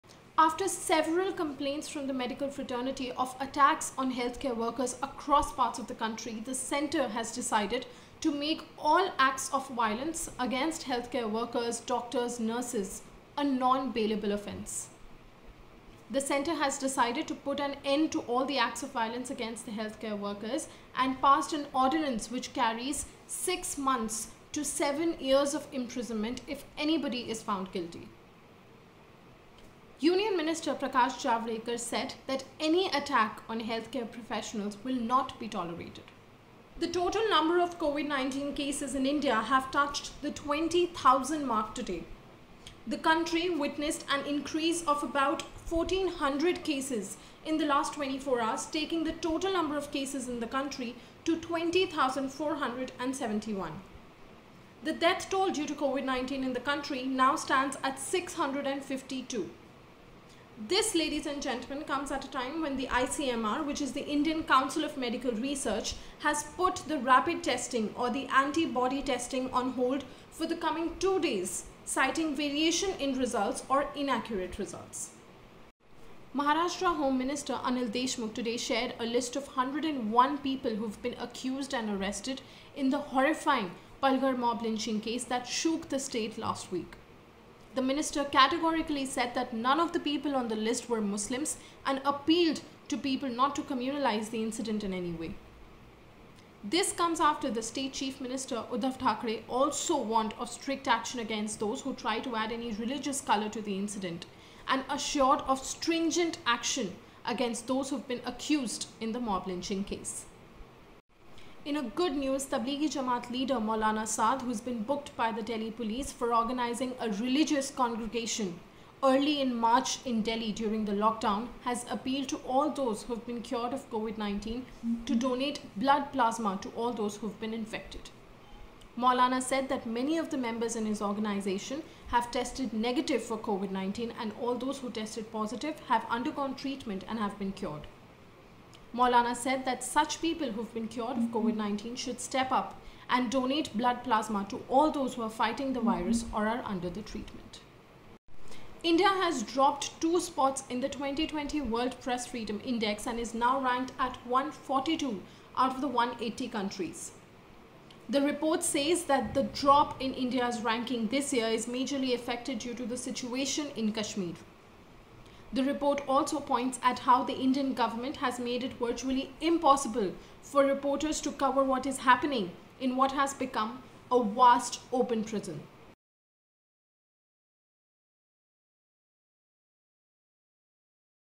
Top News Of The Day
Top five news that deserve your attention today, presented by Navya Singh